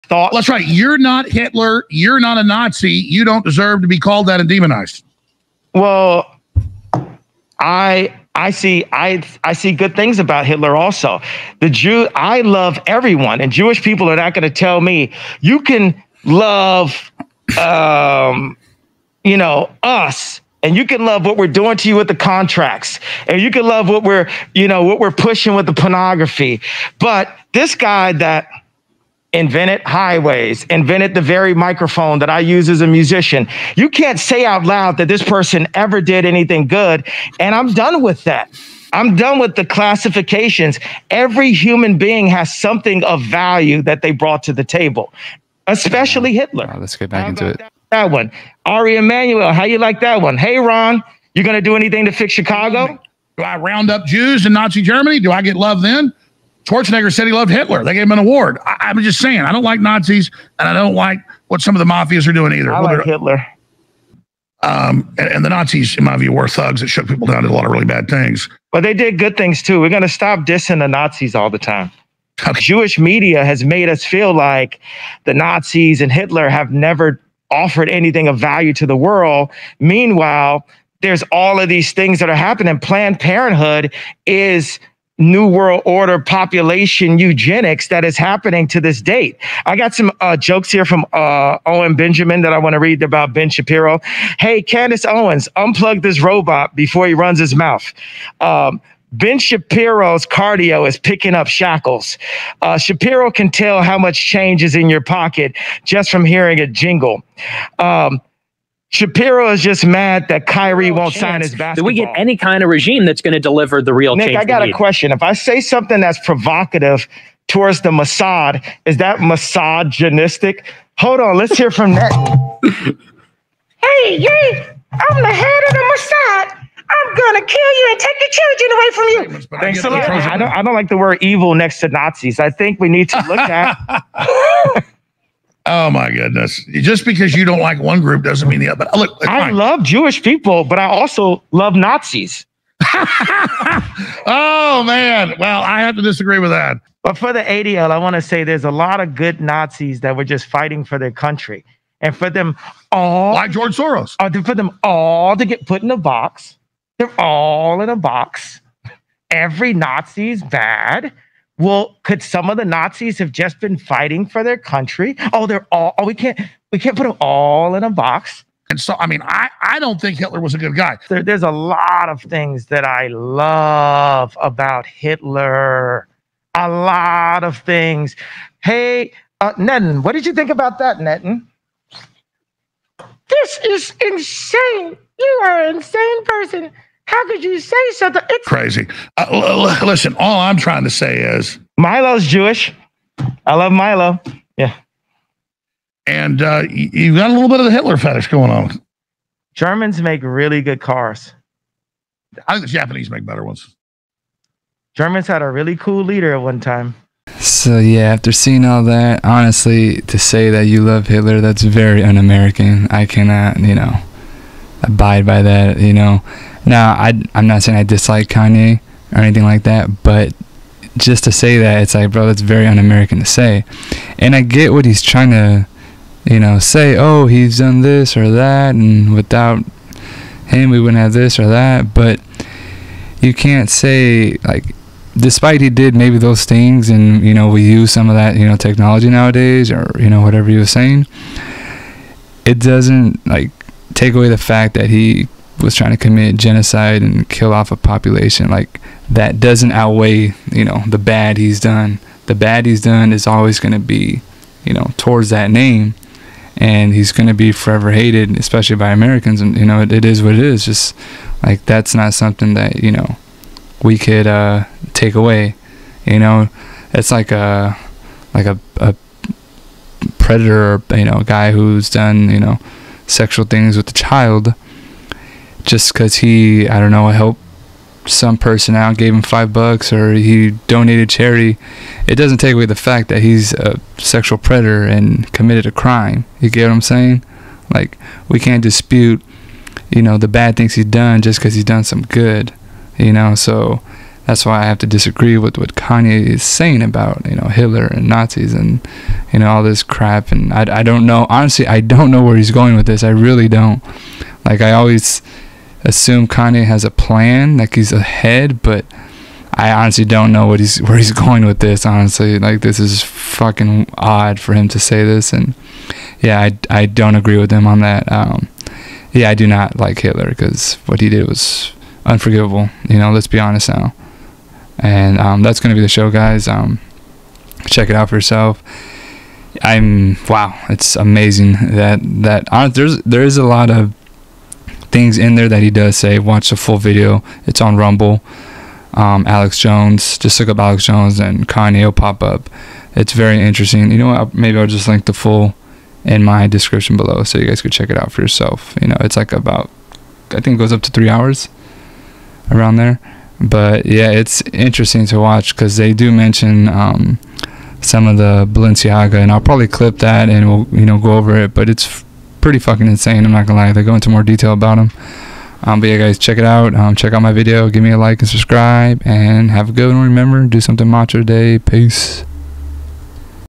Does Kanye West Have a "HITLER FETISH"? THAT'S UN-AMERICAN
Does Kanye West Have a "HITLER FETISH"? THAT'S UN-AMERICAN in my opinion check it out here or on rumble.